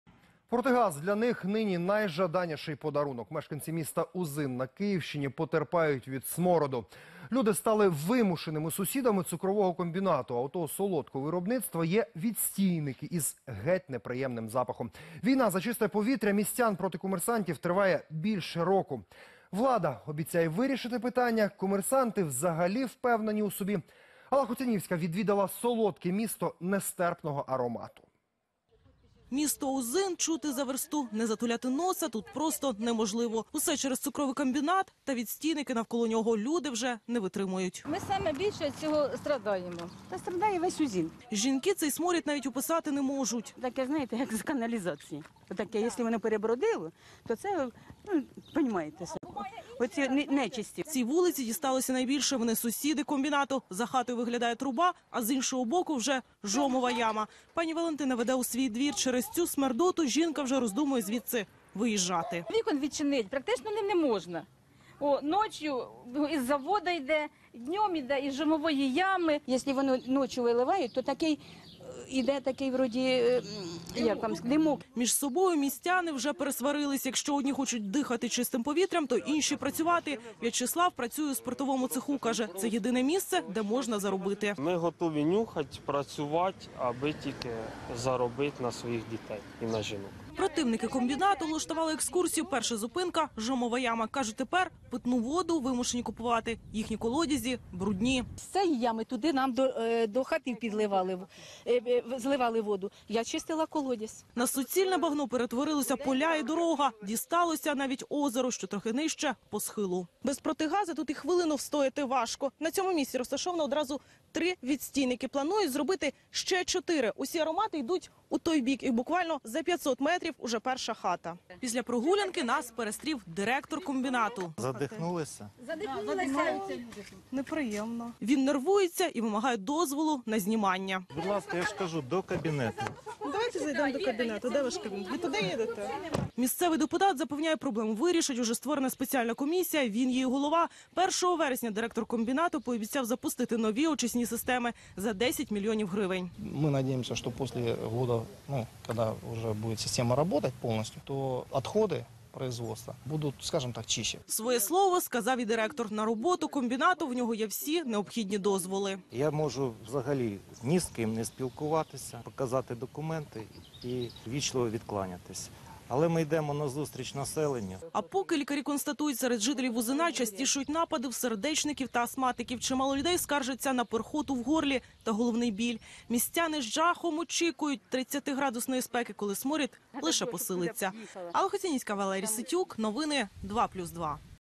Задуха на Узині
Протигаз нині для них найкращий подарунок. Мешканці селища Узин на Київщині потерпають від смороду.
Люди стали вимушеними сусідами цукрового комбінату. А у того солодкого виробництва є відстійники із геть не приємним запахом.
Війна за чисте повітря містян проти комерсантів триває більше року. Влада обіцяє вирішити питання, комерсанти взагалі впевнені у собі.